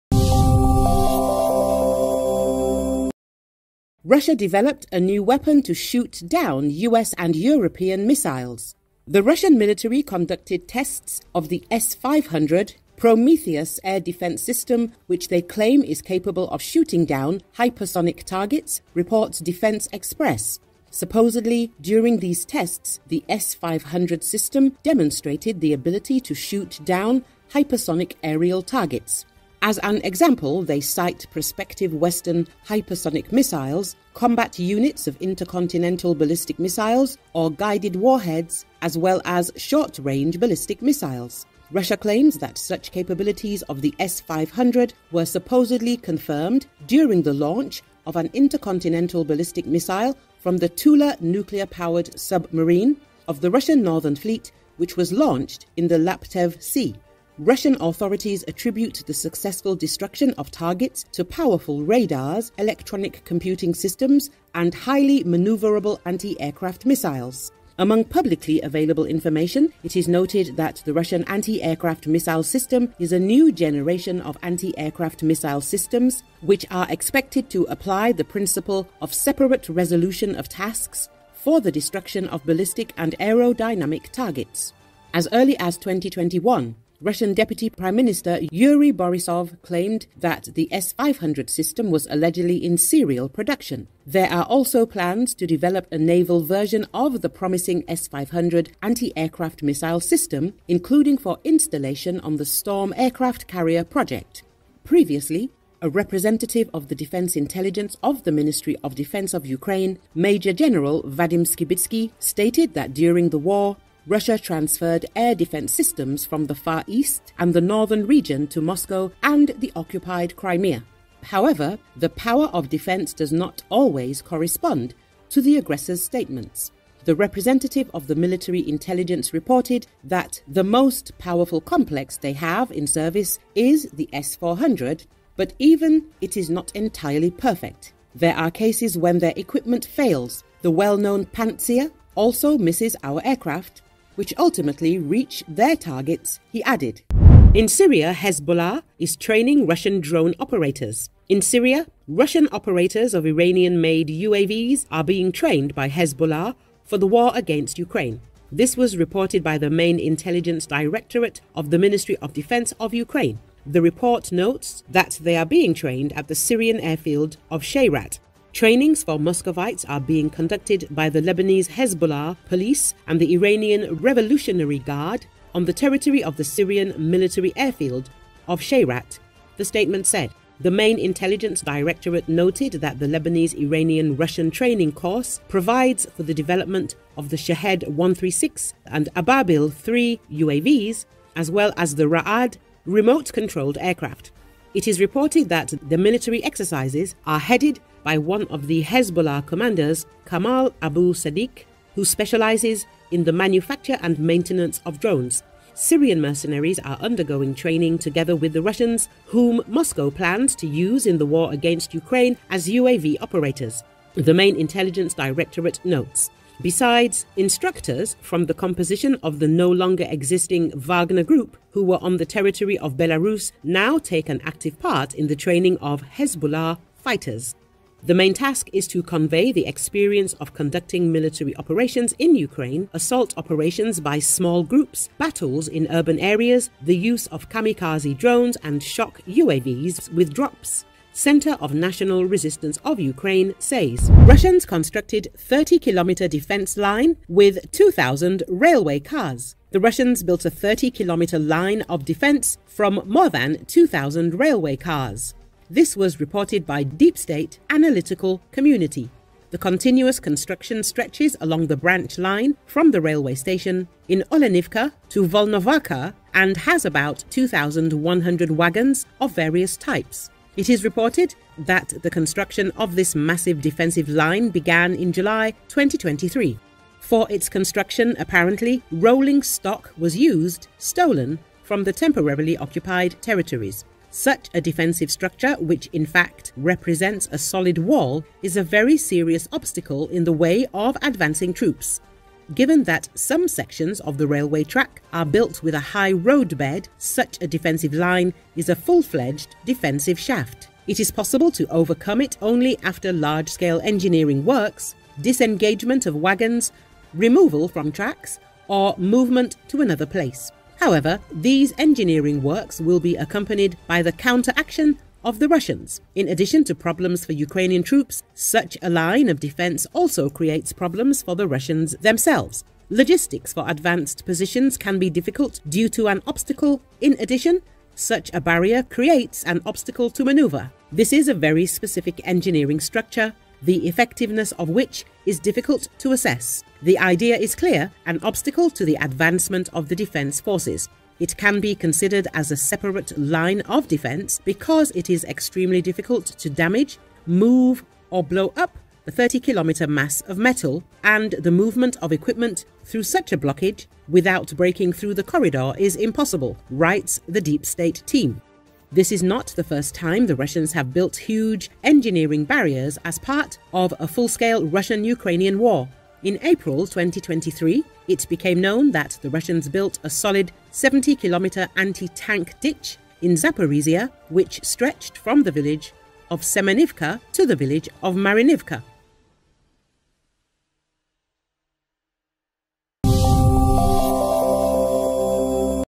Russia developed a new weapon to shoot down US and European missiles
The Russian military conducted tests of the S-500 Prometheus air defense system, which they claim is capable of shooting down hypersonic targets, reports Defense Express.
Supposedly, during these tests, the S-500 system demonstrated the ability to shoot down hypersonic aerial targets. As an example, they cite prospective Western hypersonic missiles, combat units of intercontinental ballistic missiles, or guided warheads, as well as short-range ballistic missiles.
Russia claims that such capabilities of the S-500 were supposedly confirmed during the launch of an intercontinental ballistic missile from the Tula nuclear-powered submarine of the Russian Northern Fleet, which was launched in the Laptev Sea.
Russian authorities attribute the successful destruction of targets to powerful radars, electronic computing systems, and "highly maneuverable anti-aircraft missiles."
Among publicly available information, it is noted that the Russian anti-aircraft missile system is a new generation of anti-aircraft missile systems, which are expected to apply the principle of separate resolution of tasks for the destruction of ballistic and aerodynamic targets.
As early as 2021, Russian Deputy Prime Minister Yuri Borisov claimed that the S-500 system was allegedly in serial production. There are also plans to develop a naval version of the promising S-500 anti-aircraft missile system, including for installation on the Storm aircraft carrier project.
Previously, a representative of the Defense Intelligence of the Ministry of Defense of Ukraine, Major General Vadym Skibitskyi, stated that during the war, Russia transferred air defense systems from the Far East and the Northern Region to Moscow and the occupied Crimea. However, the power of defense does not always correspond to the aggressor's statements. The representative of the military intelligence reported that the most powerful complex they have in service is the S-400, but even it is not entirely perfect.
"There are cases when their equipment fails. The well-known Pantsir also misses our aircraft, which ultimately reach their targets," he added.